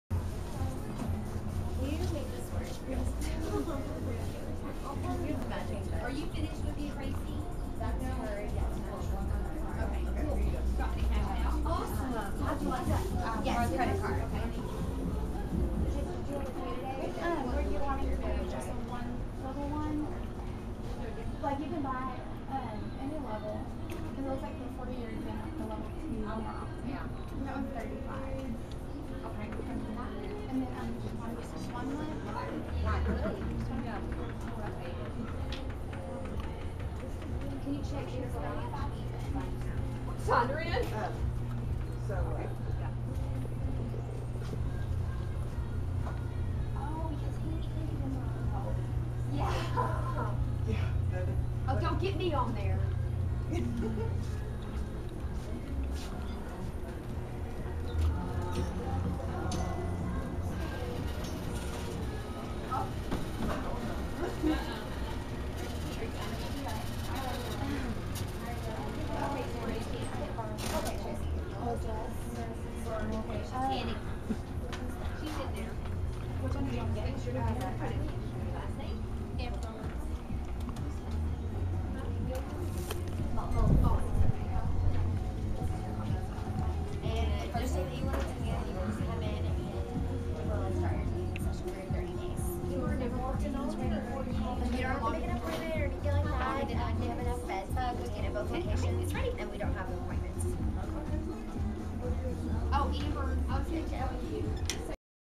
ELITE PHYSIQUE - "Spring Break Spectacular"
Some fun video footage of Elite Physique's "Spring Break Spectacular" sale at the Knickerbocker location. Recorded on Tuesday, March 12, 2010.